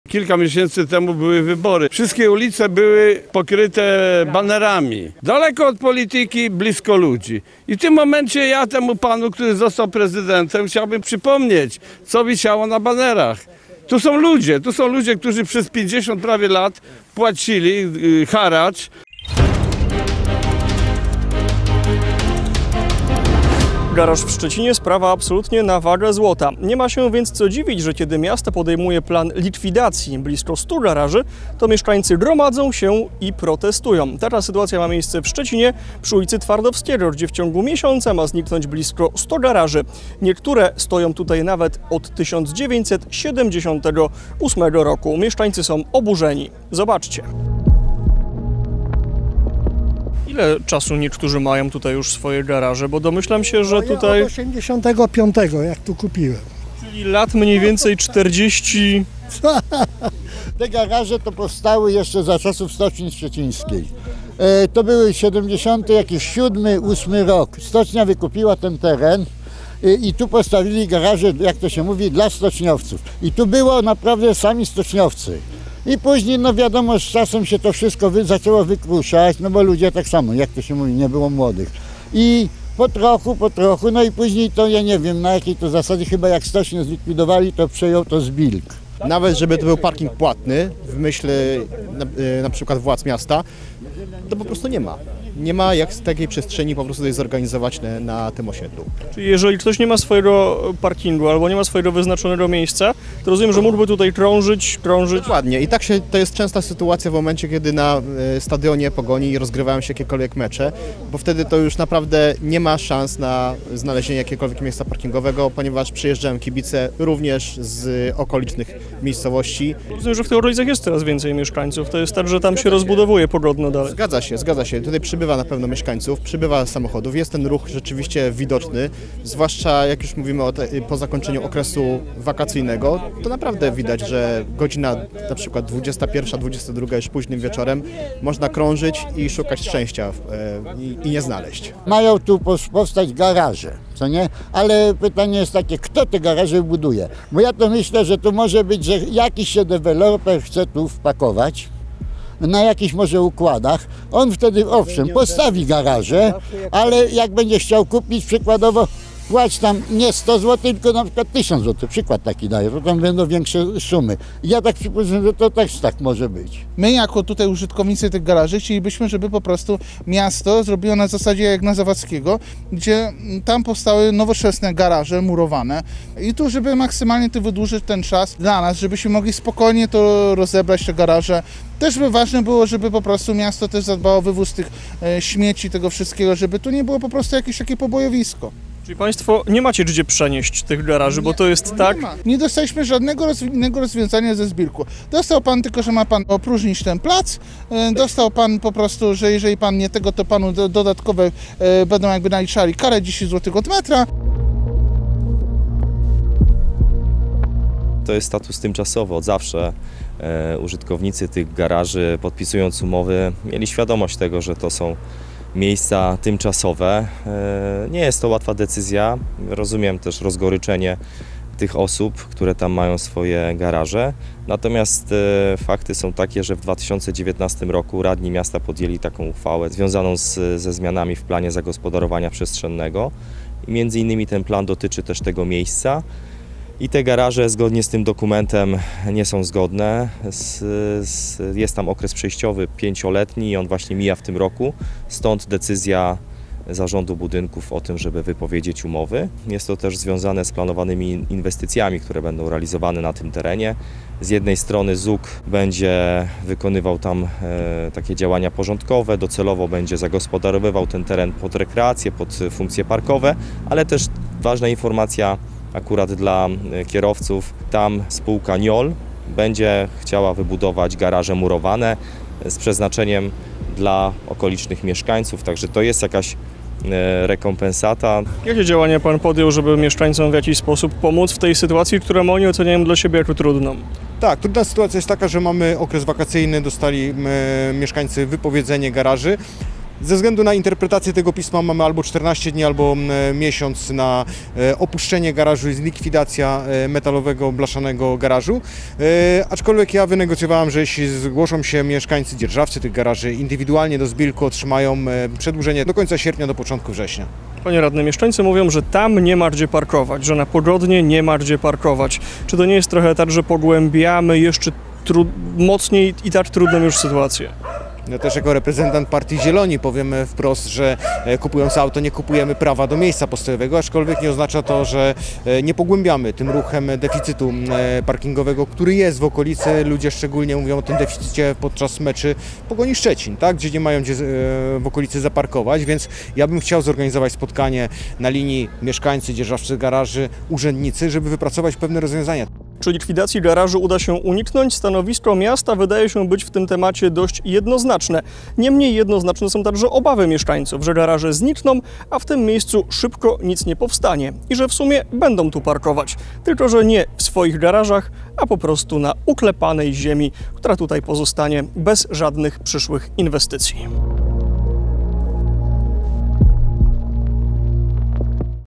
100 garaży zniknie z Pogodna w Szczecinie. Mieszkańcy są oburzeni | INTERWENCJA! wSzczecinie.pl
Nawet 100 garaży ma zniknąć w najbliższych dniach z ul. Twardowskiego w Szczecinie. Mieszkańcy są oburzeni, bo Zarząd Budynków i Lokali Komunalnych wypowiada im umowę z zaledwie miesięcznym okresem wypowiedzenia. Problemem jednak nie jest sam tryb wypowiedzenia umowy najemcom, co brak możliwości parkowania na Pogodnie. – Będziemy parkować na klepiskach albo szukać miejsc na osiedlach. Tutaj nie ma miejsca nawet na parking płatny – mówią nam mieszkańcy.